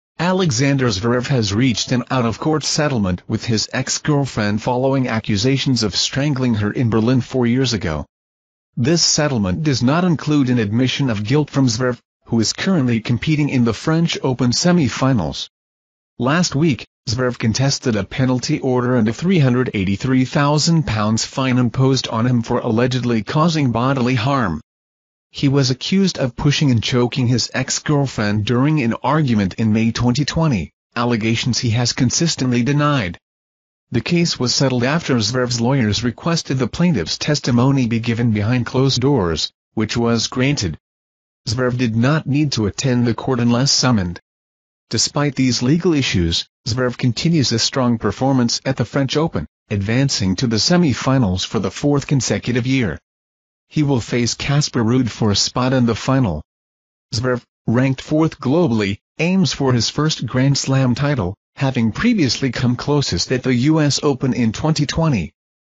Alexander Zverev reaches out-of-court settlement after being accused of strangling his ex
SPORTNEWS AlexanderZverev FrenchOpen Alexander Zverev remains in contention to win this year's French Open title.